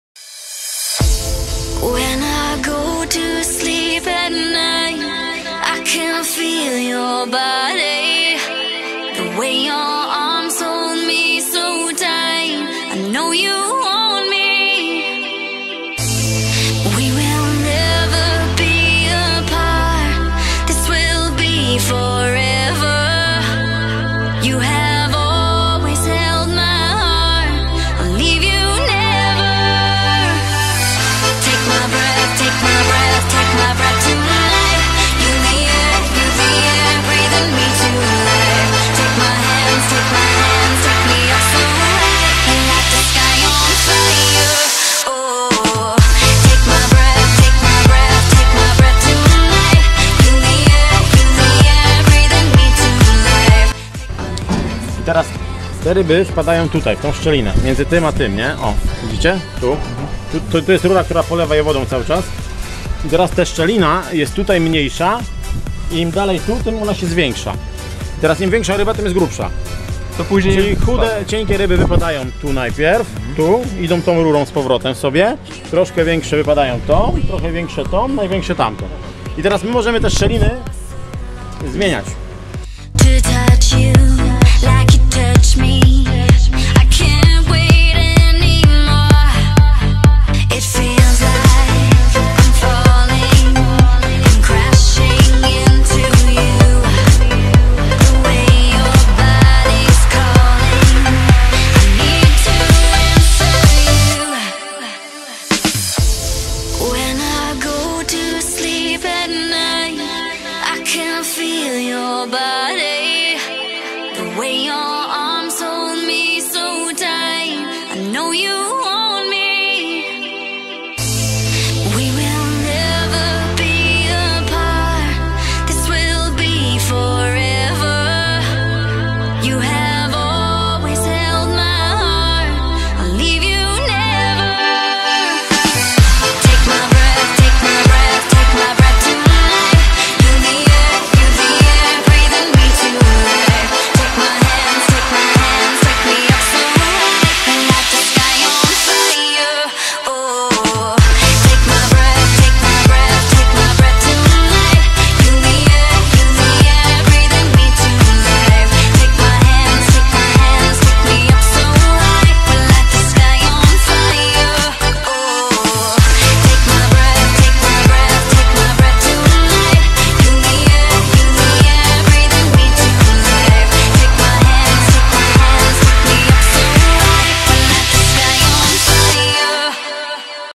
Z wizytą u BarthAgro |Stawy Pstrągowe Upiłka|Prezentacja Gospodarki| Pomorze d^_^b
Siemanko
Oglądać w HD!!!
Dzisiaj byłem wraz Dawidem Agriculture u BarthAgro :)
Podziękowania dla Bartka i Kacpra za pokazanie i oprowadzenie po stawach i gospodarce :)
Stawy rybne znajdują się w małej wiosce Upiłka.
Natomiast Gospodarstwo niedaleko nich.
Na Stawach są chodowane tylko i wyłącznie pstrągi tęczowe :)